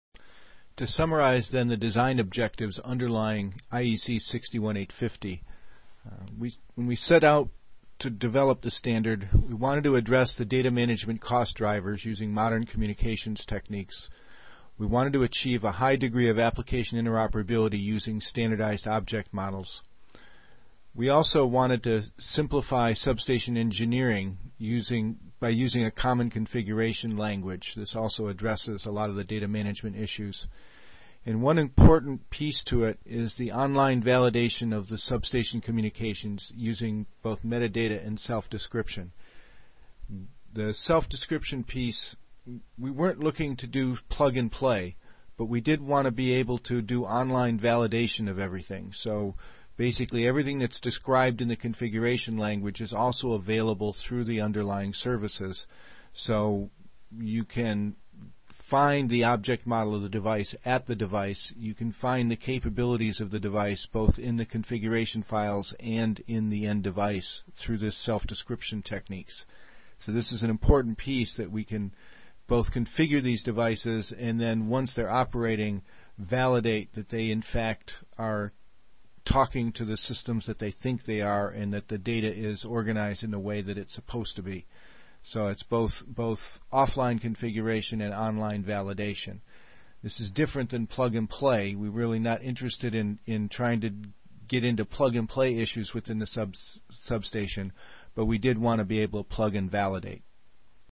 61850 Intro 5 IEC 61850 Design Objectives
This video is part of a free Introduction to IEC 61850 video course.